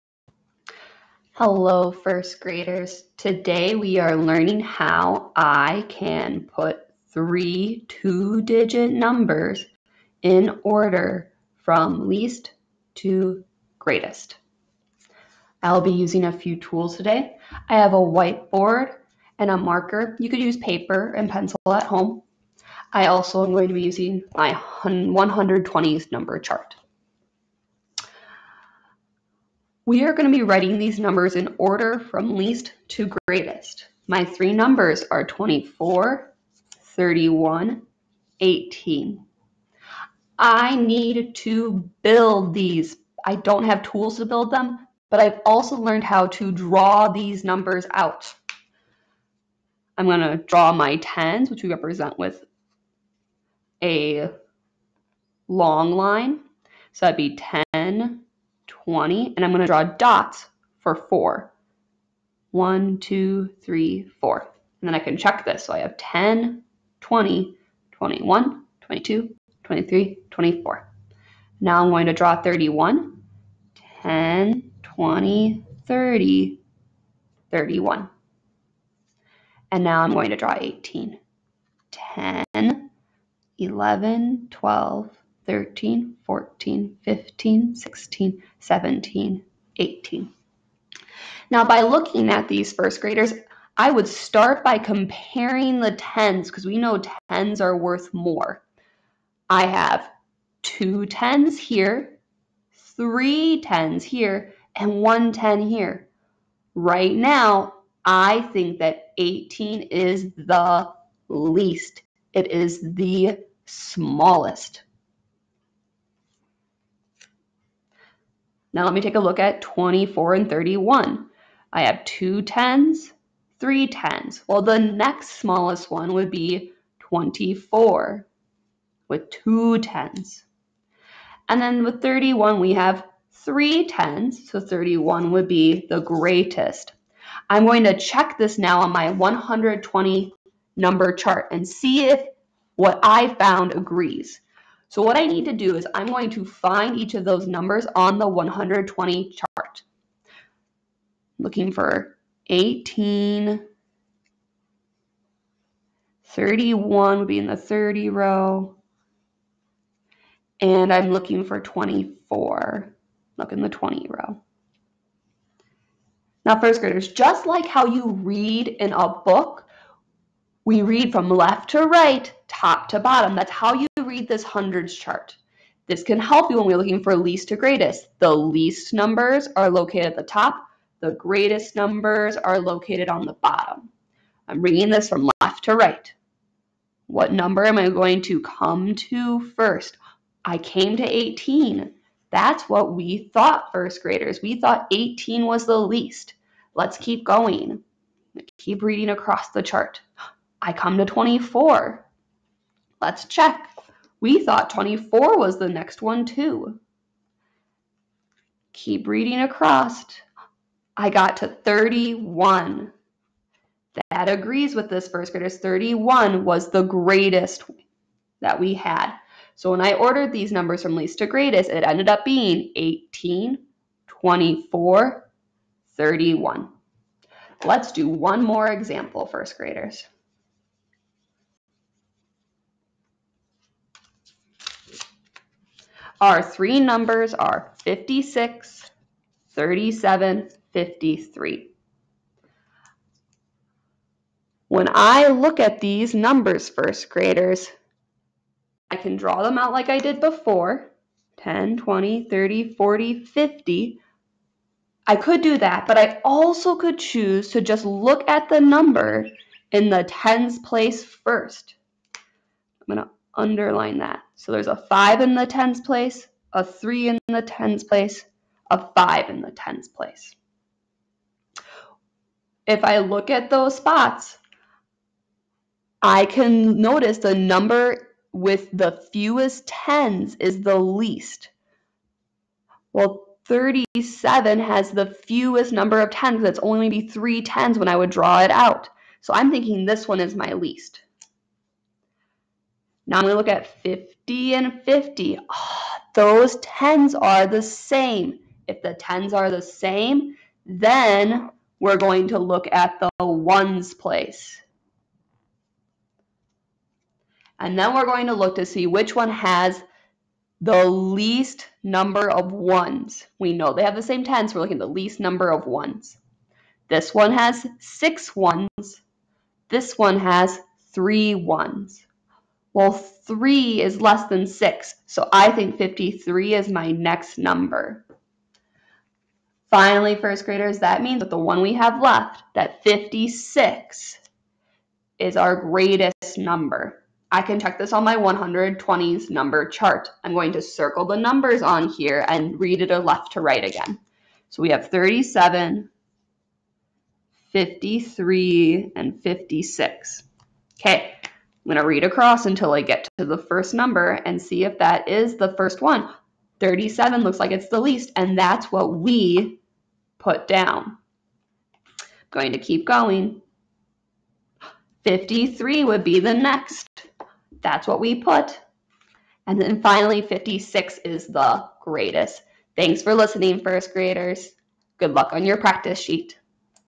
Ordering Numbers Least to Greatest
This video models how to order numbers from least to greatest. The tools used were a white board & marker (could use paper & pencil) and a 120s Number Chart.